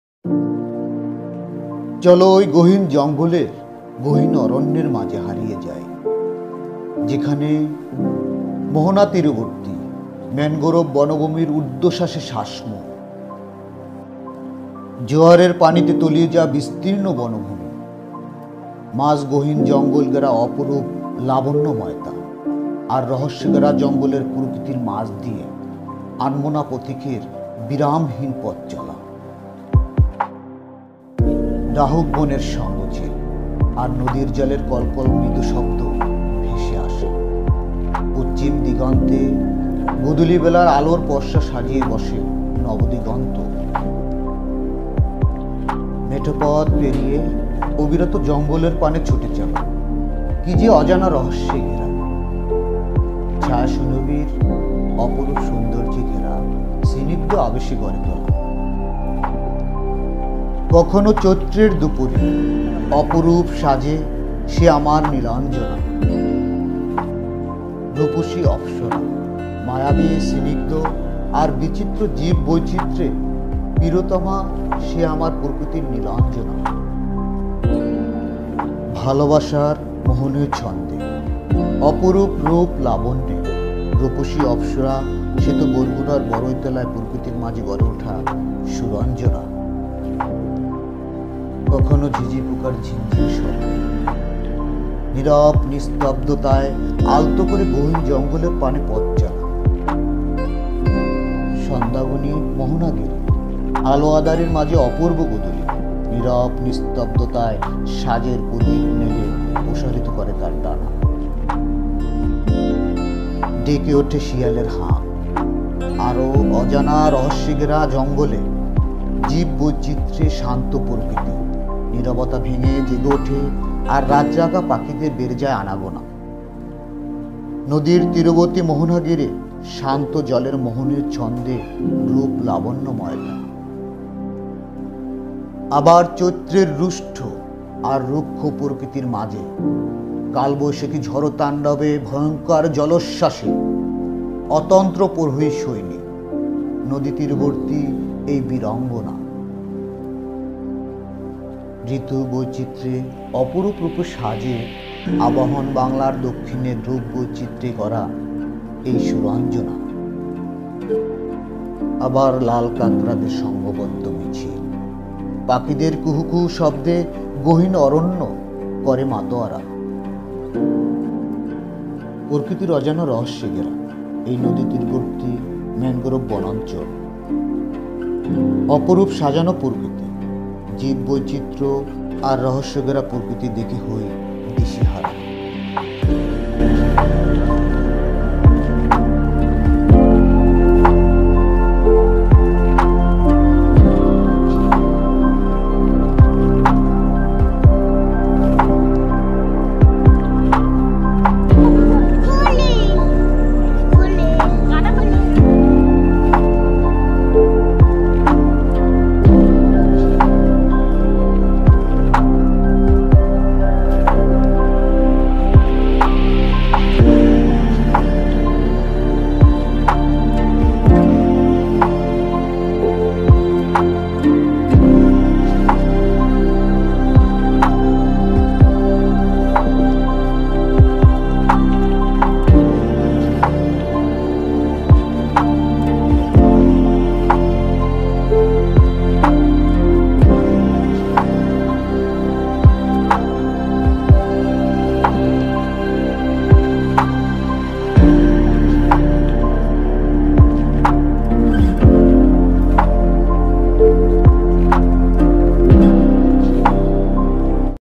সুরন্জনা | প্রকৃতির মাঝে সাজানো রুপ লাবন্যে গড়া অপরুপ
Let's get lost in the deep forest of that deep forest.
Where the mangrove forest breathes deeply.
Extensive forests submerged in tidal waters,
In the middle of the deep jungle surrounded by wonderful beauty and the nature of the jungle surrounded by mystery, the restless path of the mindless traveler.
The conch shells of the Dahuk forest and the murmur of the murmur floating in the river water,
Twilight is set on the western horizon,
Crossing the sweet path and running to the end of the forest,
What is it like to build a shadow surrounded by unknown mysteries in a deep, gentle obsession,
Nilanjana, sometimes in the afternoon of Chaitra, in beautiful attire,
Rupshi apsara magical smooth and diverse biodiversity she is my beloved Nilanjana,
Rupshi Apsara in the charming rhythm of love, Suranjana formed in the middle of nature on the big floor of Barguna.
Sometimes the squeaking sound of crickets is lost in the stillness of the deep forest,
In the evening, there is a wonderful twilight around the estuary.
Its wings spread out in silent silence,
The croaking of the fox called out in the evening.
In the forest surrounded by more unknown mysteries, the calm, soothing and captivating nature of the biodiversity breaks the silence and awakens the nightingales, Anagona,
The enchanting rhythm of the calm water around the estuary on the banks of the river.
Again, in the midst of the angry and rugged nature of Chaitra, this heroic heroine on the banks of the river, the vigilant watchman, is tormented by the Kalvaishakhi storm.
This suranjana is formed in the midst of nature, which is beautifully decorated with the variety of seasons.
Once again an organized procession of red crabs,
The sound of birds chirping in the deep forest intoxicated.